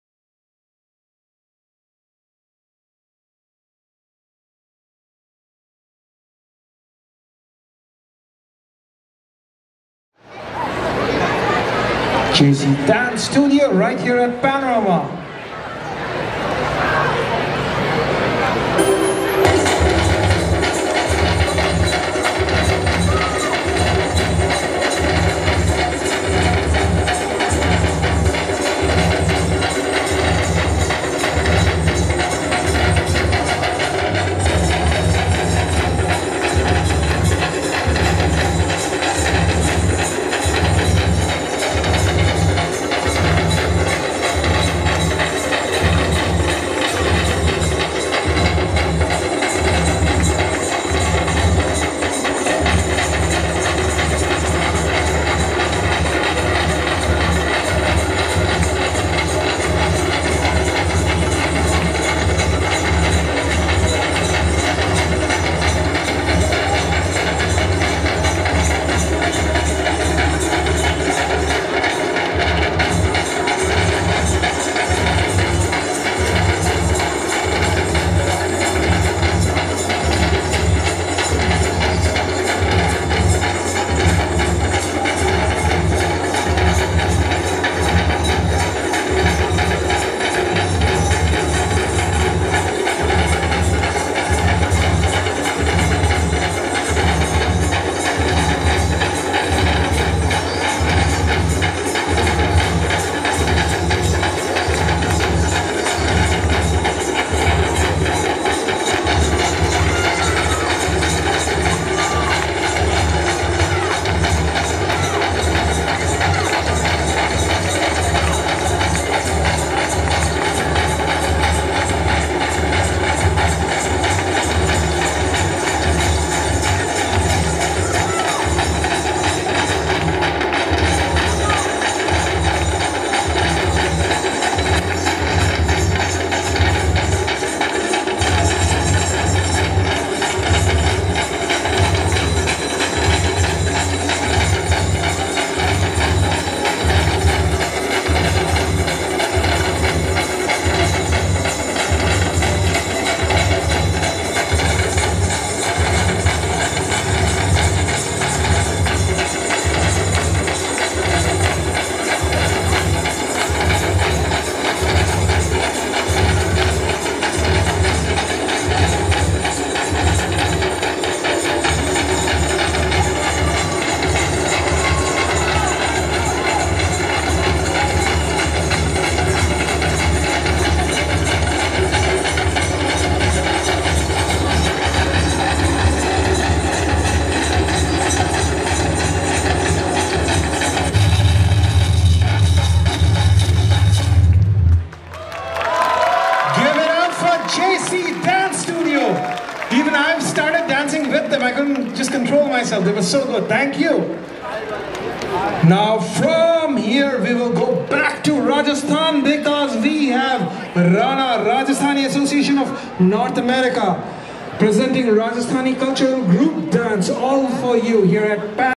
Folk Dance representing TamilNadu @ Panorama India - Independence Day 2023
Representing Tamil Nadu at Panorama India 2023 event, Nathan Phillips Square, Toronto.